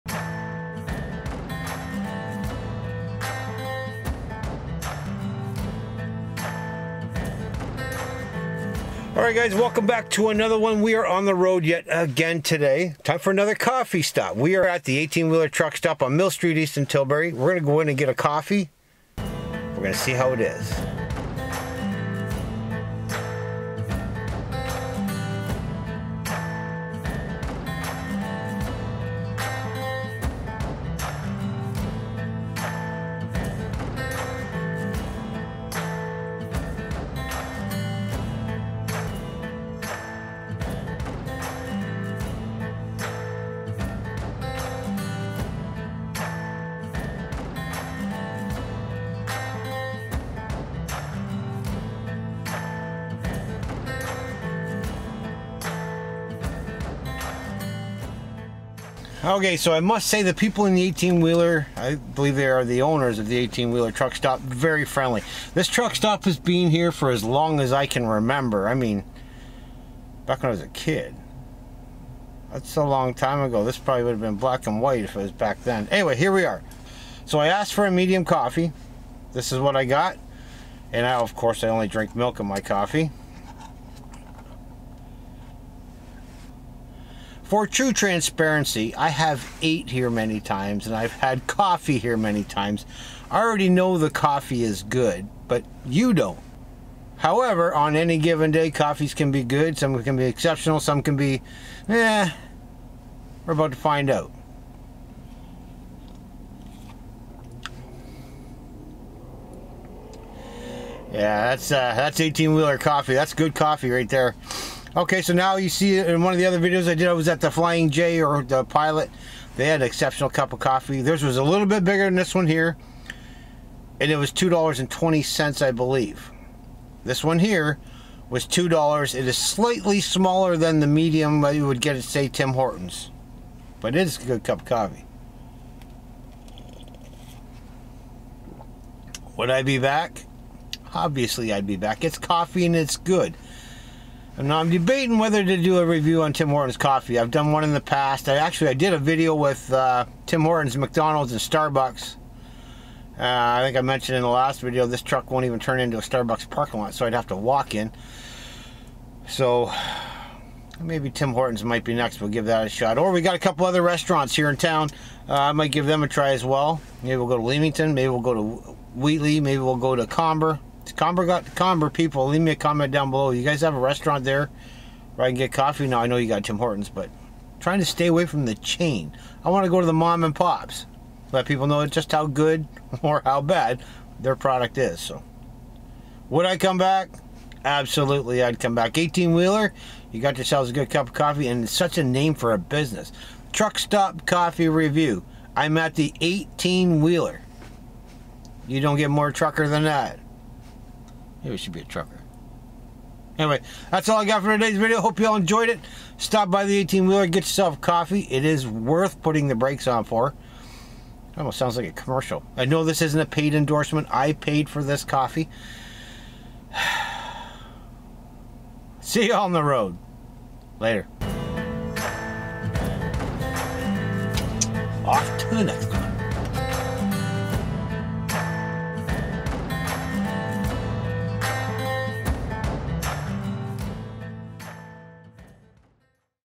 Truck Stop Coffee at the 18 Wheeler in Tilbury Ontario
We continue our truck stop coffee review with a stop at the 18 Wheeler in Tilbury Ontario. Is it worth it?